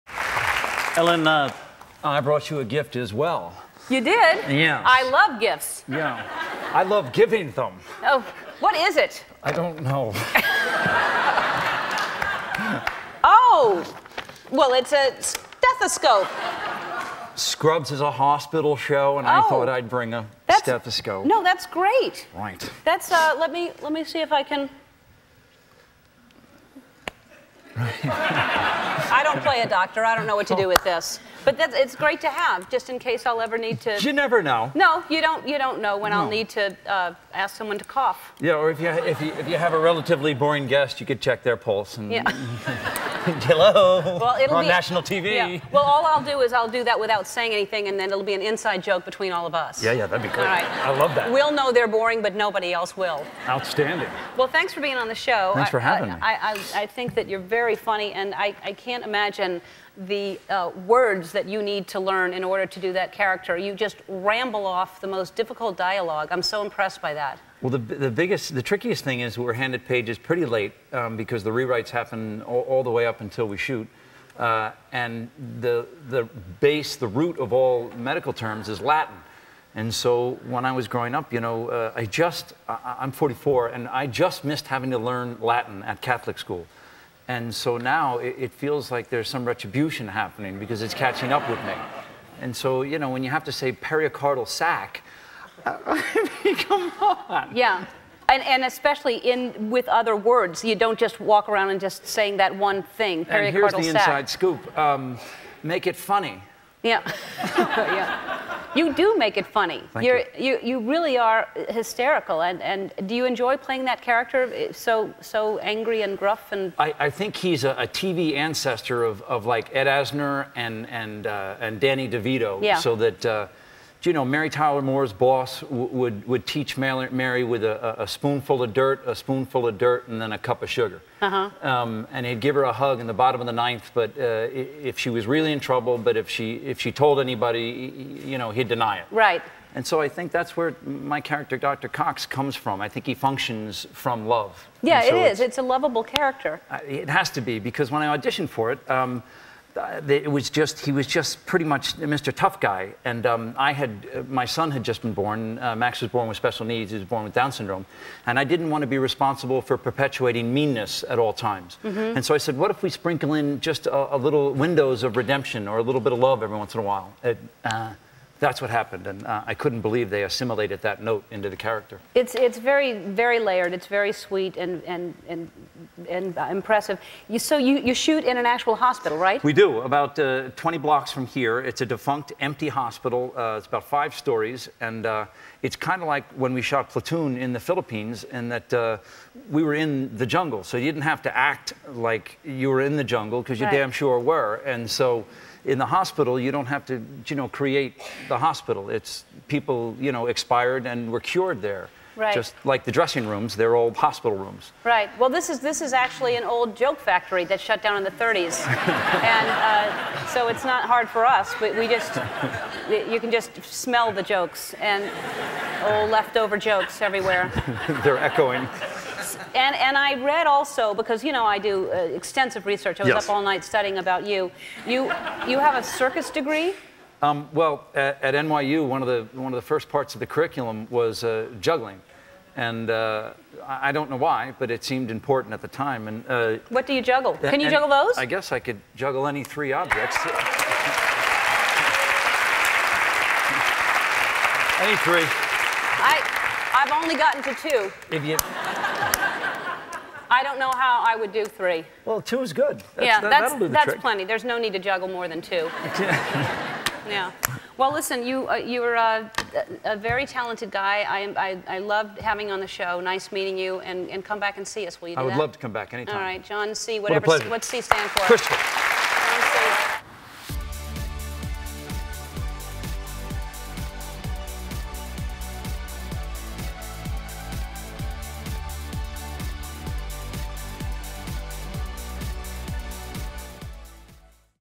'Scrubs’ Star John C. McGinley on His Circus Degree
John C. McGinley from “Scrubs” discusses learning doctor lingo, filming in an actual hospital, and his circus degree.